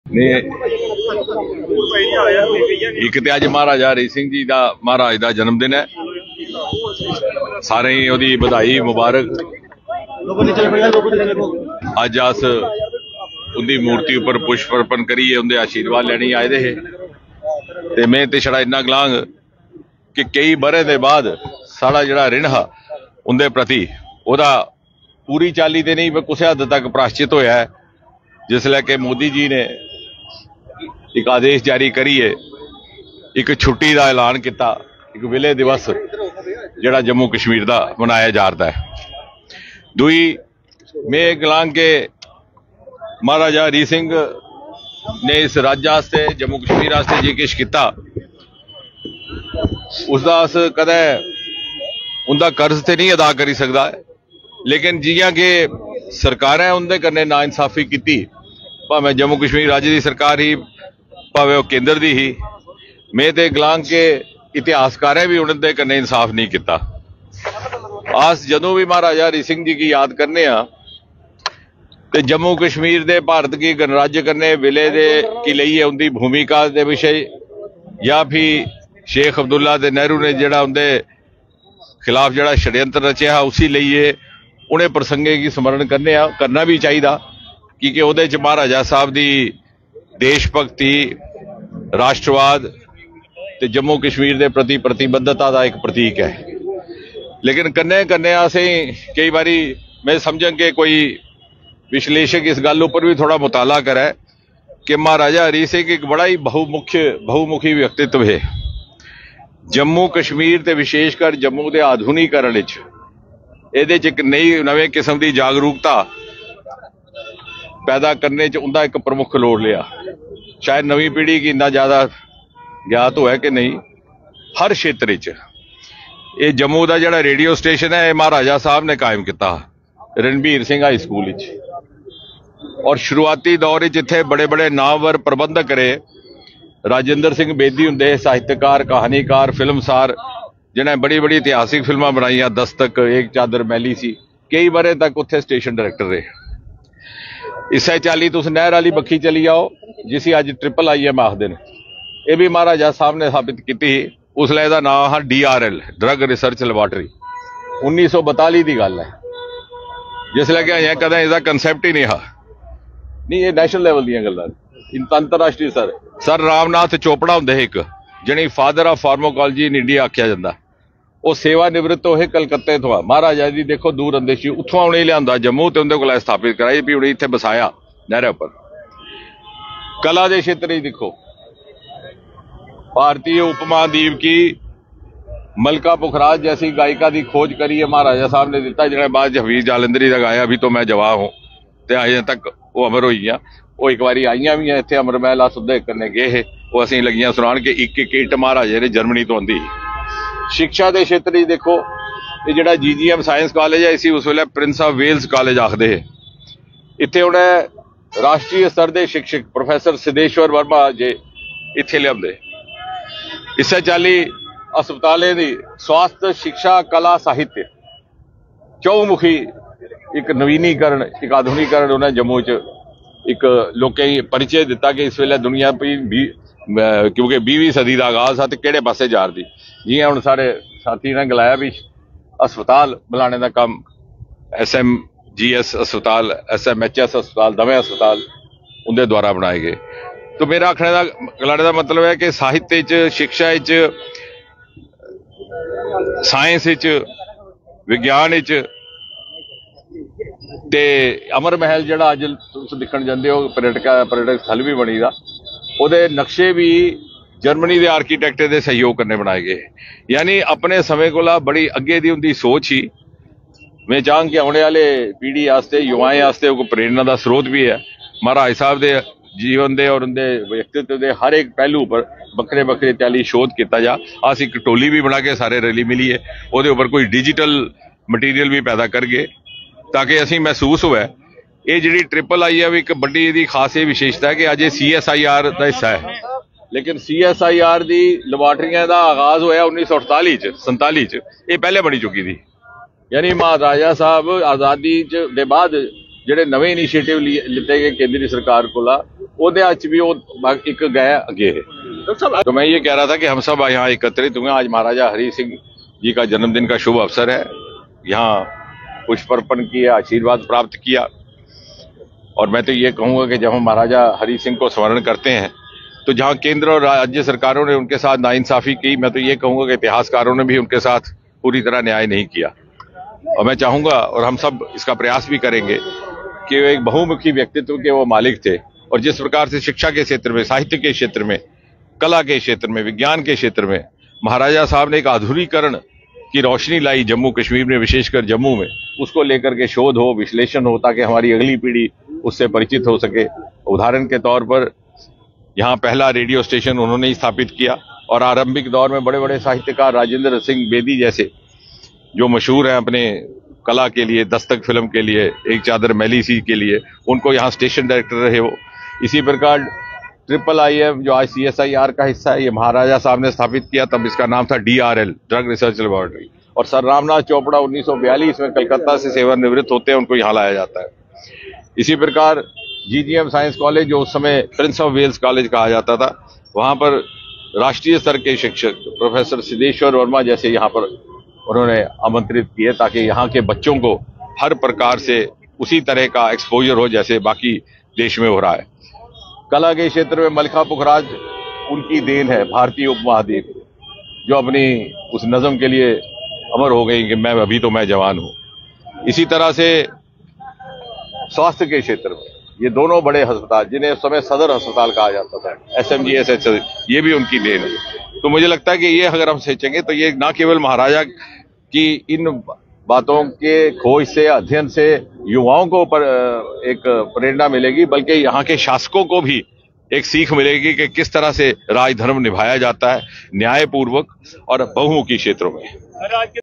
Interaction with media after paying obeisance to MaharajaHariSingh on his 129th Birth Anniversary.
Jammu